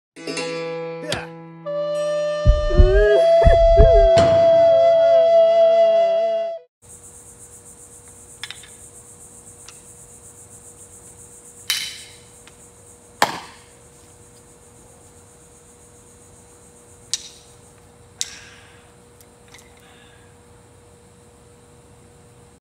Nunchuck Bottle Cap Challenge
Bottle cap challenge with a beer bottle and a pair of nunchucks because. America!